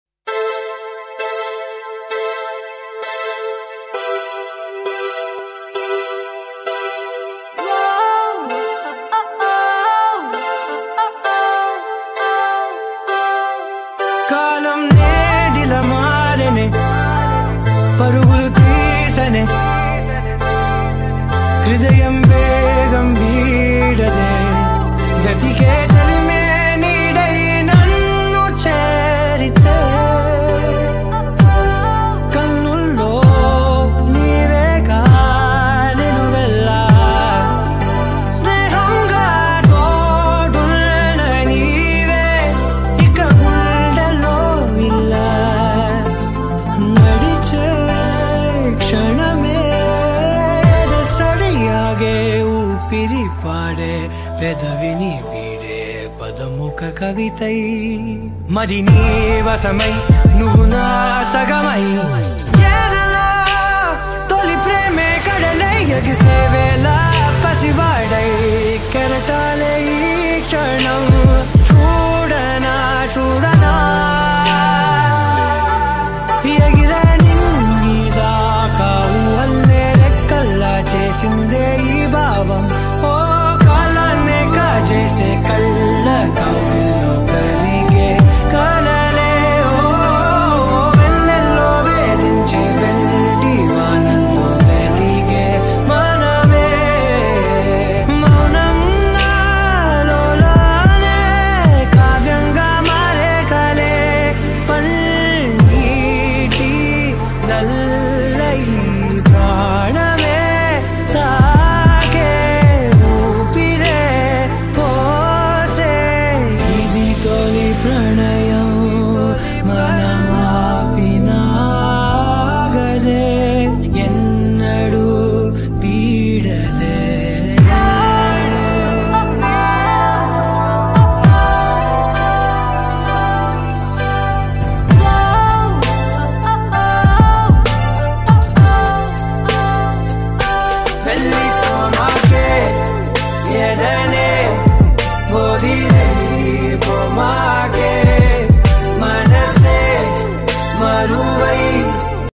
📍💚 Youth Festival - 2022 Diaries Aurora NSS💕💚📍
Aurora NSS volunteers attended Youth Festival 2022 held at Kakatiya Medical College Warangal.